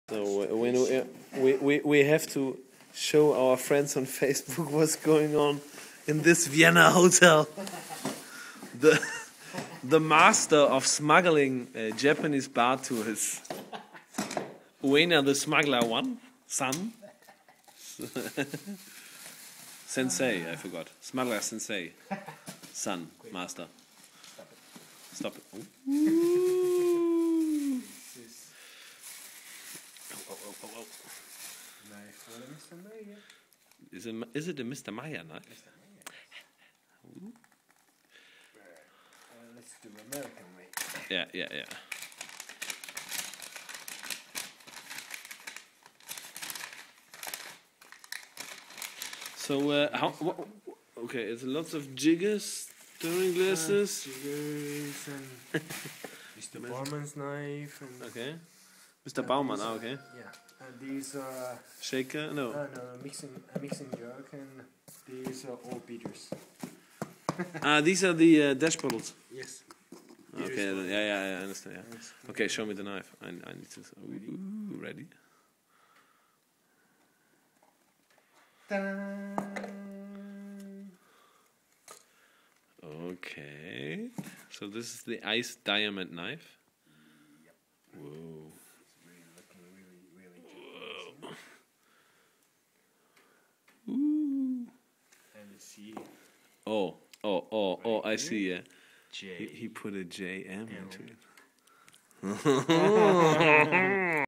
Ueno_the_Smuggler
The Smuggler delieverd a whole selection of finest japanese Bar Tools for Friends of LE LION